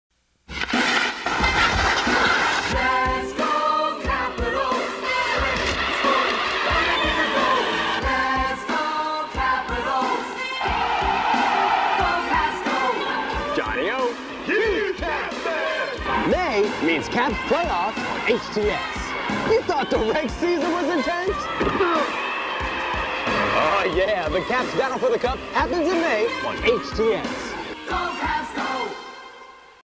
Johnny O commercial@ Cap Center
Johnny O, played by John ONeal, represented the ultimate sports fan, who loved his teams, and lived for sports. Johnny O commercials ran from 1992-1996 on Home Team Sports, promoting regional sports teams like the Baltimore Orioles, Washington Bullets, and Washington Capitals.